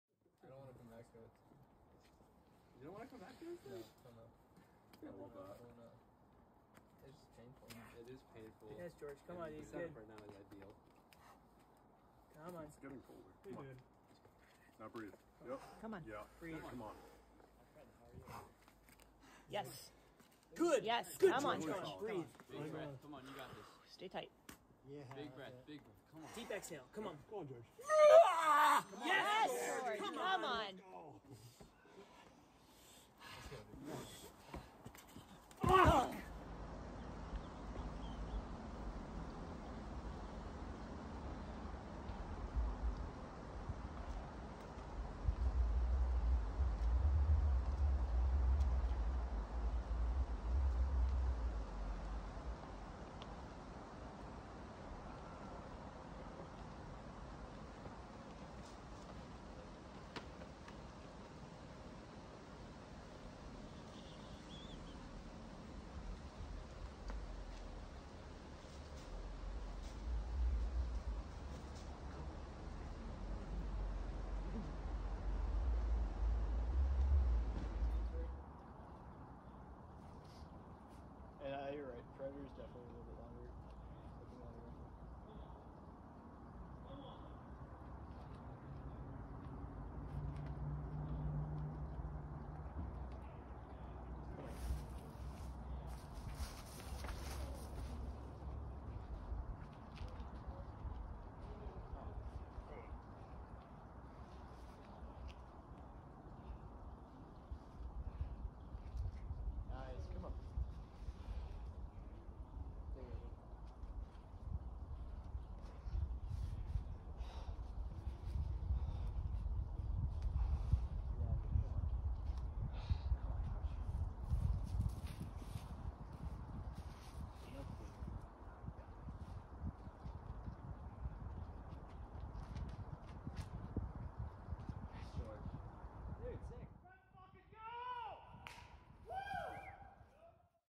Speed of Life V10
I first heard of this boulder in January 2021. I had just moved to New England at that point, and was just beginning to learn about the climbing community and areas. I had just done my third double digit boulder ever, so hearing about this legendary boulder in Western Mass not listed in guidebooks really intrigued me but I didn’t feel ready to try it. In late 2021 I finally made my first trip to Farley, saw the boulder in person and was blown away by how beautiful it is. Two years later, I finally felt solid enough to give it a try. With the help of supportive friends, a log landing ramp and a lot of psych, I found myself on top of this dream boulder. Thank you Ronan from CRG Cambridge for keeping me safe and getting up so early to work (and send) this climb with me!